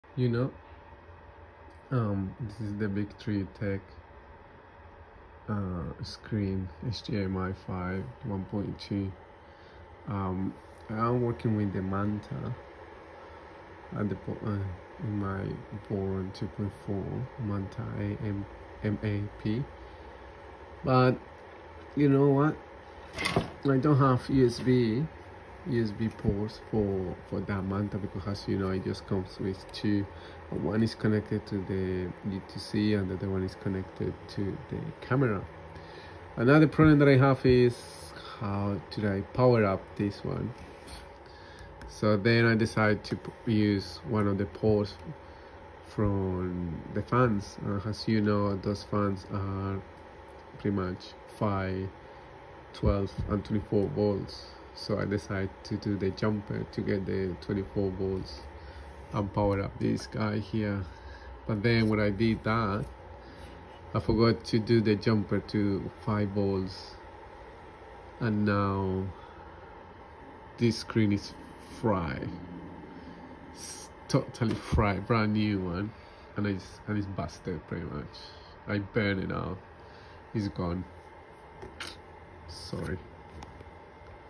BTT HDMI5 1.2
I am building the Voron 2.4 that I got from Formbot but, the M8P has only 2 USB ports. Unfortunately both are taken, and the U2C doesn't provide power over USB. I decide to use one of the M8P fan ports because it has the advantage to jumper the 5V, 12V and 24V depends on your need. However, I got distracted and forget to do the 5V jumper then. you know what happened at end.